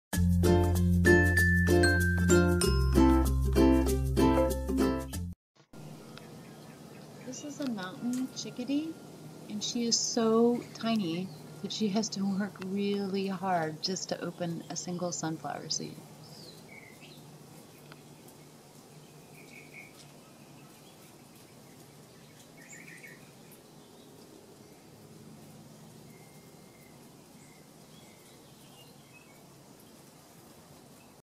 Mountain Chickadee Opening Sunflower Seed
Mountain Chickadee cracking open a sunflower seed with beak.